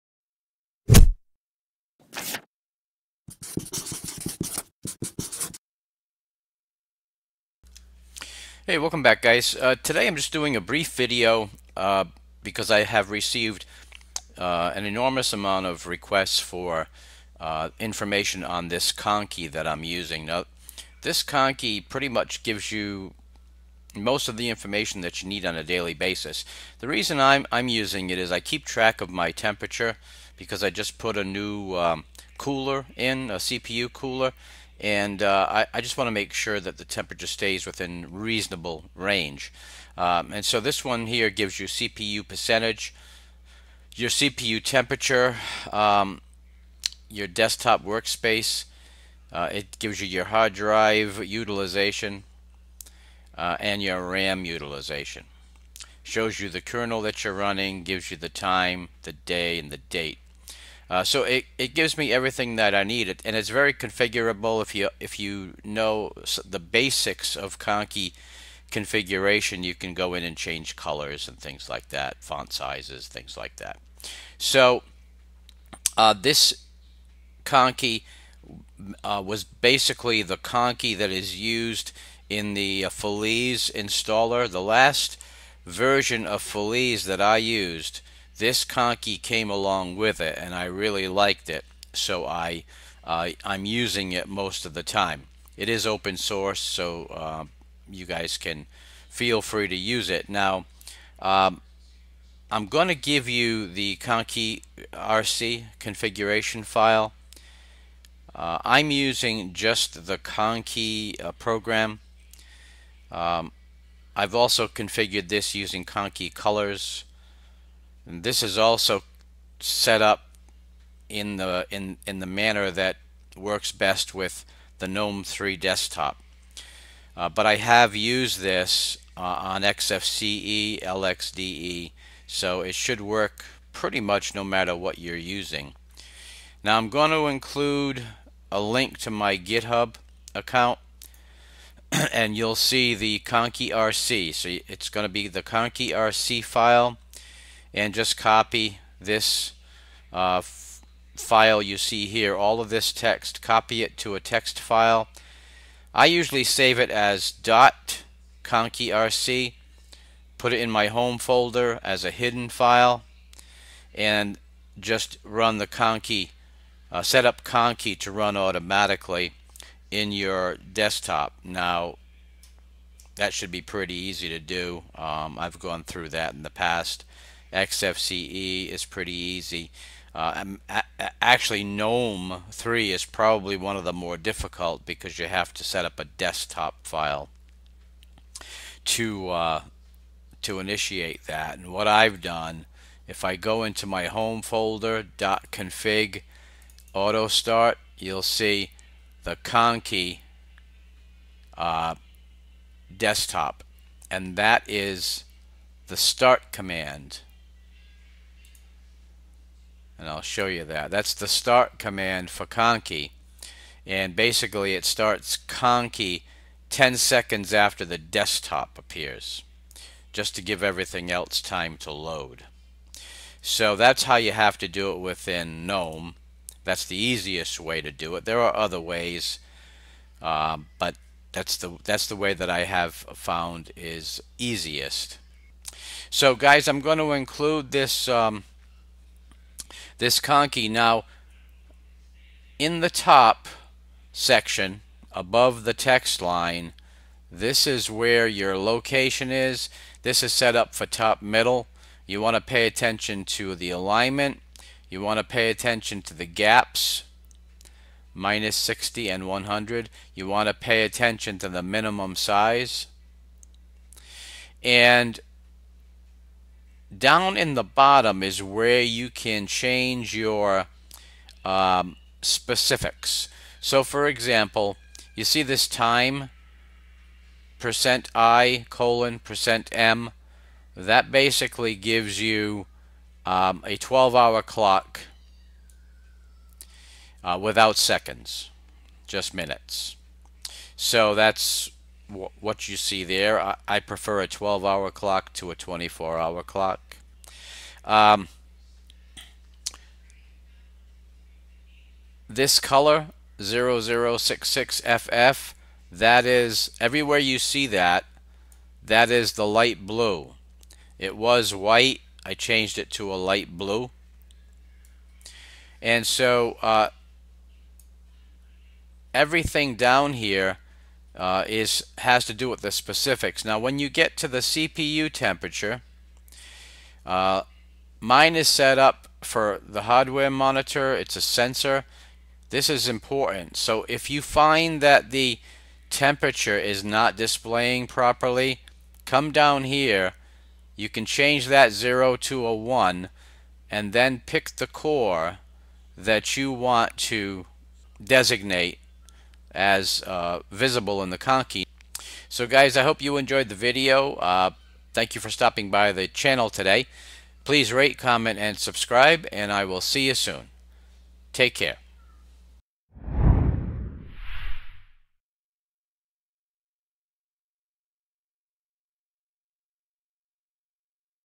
Conky - As You Requested!
In this video I provide information regarding the conky I am using. File is included. Hope you enjoy!